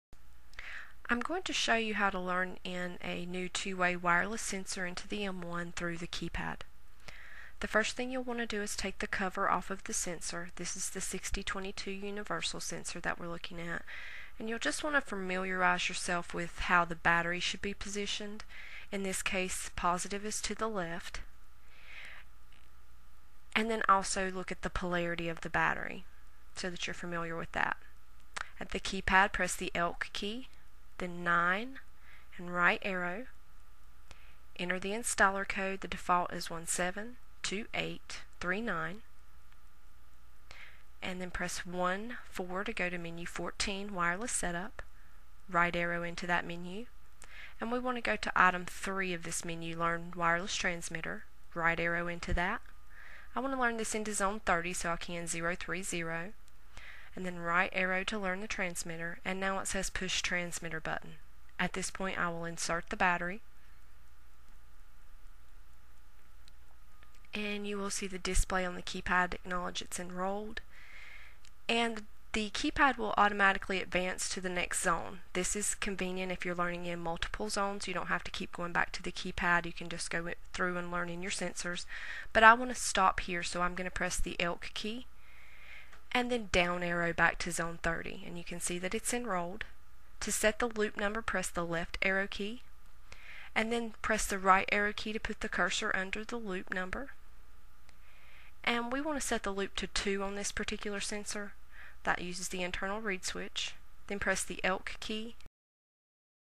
Two-Way Wireless - Enrolling through the keypad
How to enroll the ELK-Two-Way Wireless Sensors in the M1 from a keypad